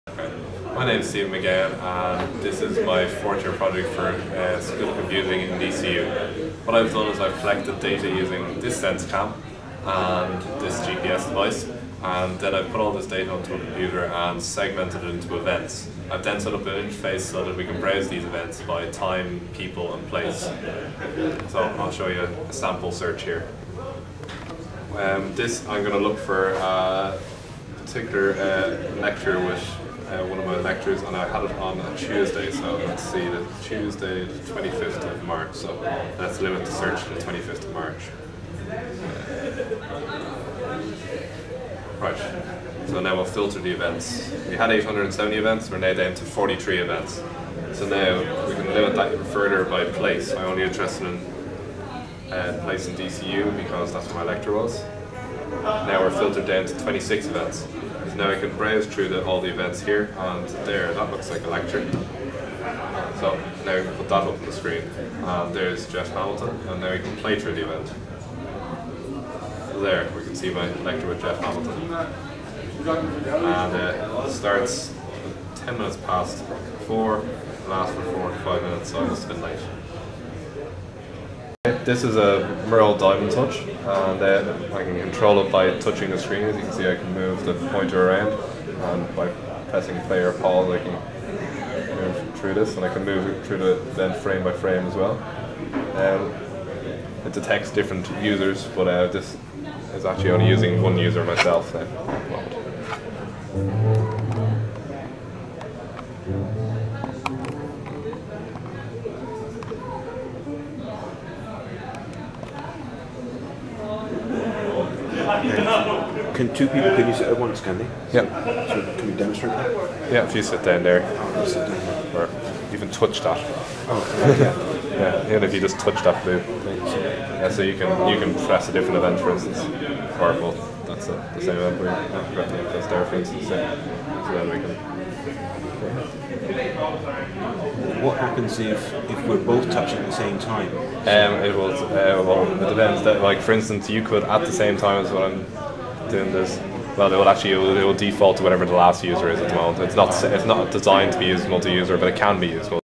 Touch Screen Interface for Browsing Life-log
A final-year project at the School of Computing, Dublin City University. A touch-screen interface for browsing images produced by a SenseCam.